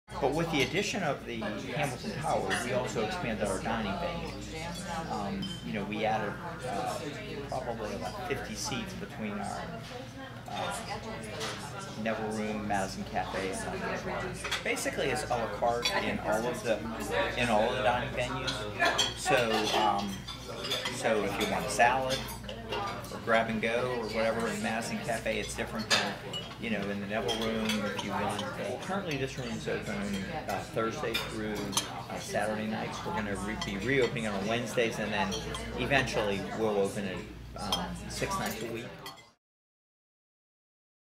Bob Kocent
Executive Director, Bob Kocent, boasts about the various options for living quarters at Providence Point.